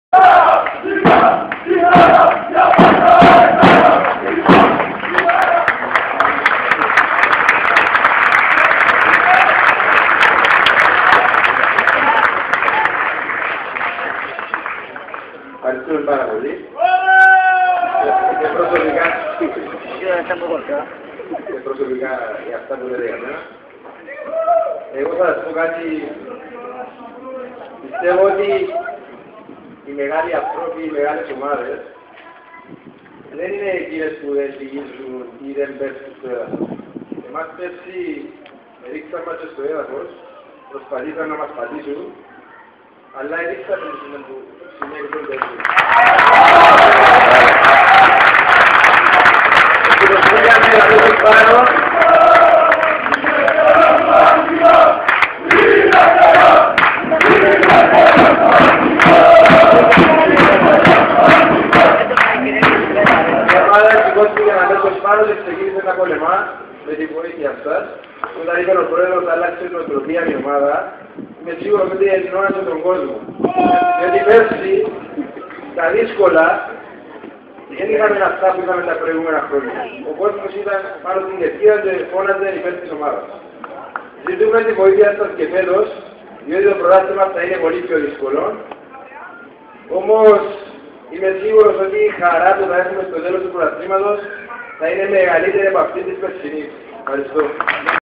Konstantinos Mina-Episimi Prwti 09/07/2008
Mina-Psixara-Gia panta AEKARA!!!  Oi dilwseis tou arxigou stin episimi prwti proponisi (09/07/08)